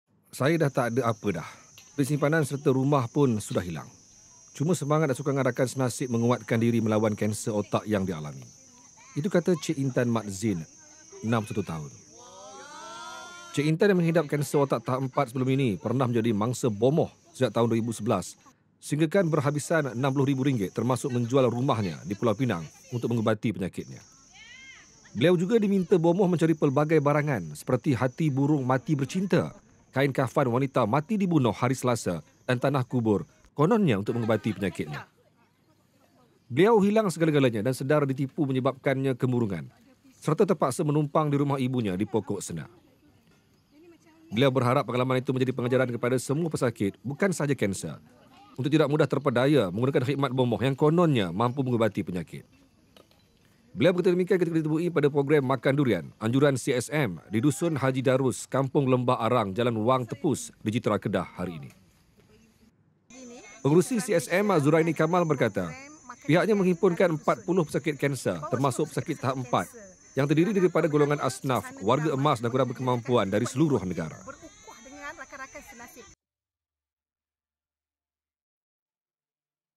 Pesakit kanser tinggal sehelai sepinggang ditipu bomoh RM60,000
"Saya dah tak ada apa dah, duit simpanan dan rumah sudah "hilang", cuma semangat dan sokongan rakan senasib menguatkan diri melawan kanser otak yang dialami," kata Che Intan Md Zain, 61.

Che Intan yang menghidap kanser otak tahap empat sebelum ini pernah menjadi mangsa bomoh sejak 2011 sehinggakan berhabis RM60,000, termasuk menjual rumahnya di Pulau Pinang untuk mengubati penyakitnya.